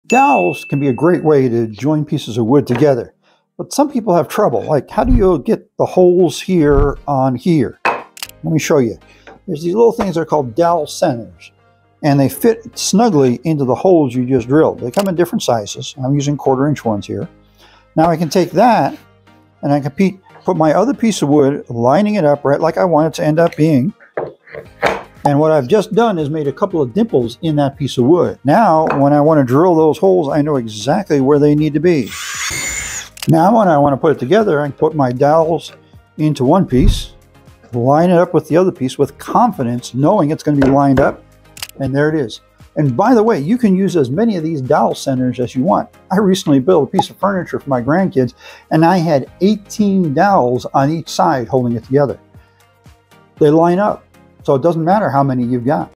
Use dowel centers to get perfect alignment every time!
Master wood joints with this essential dowel center technique.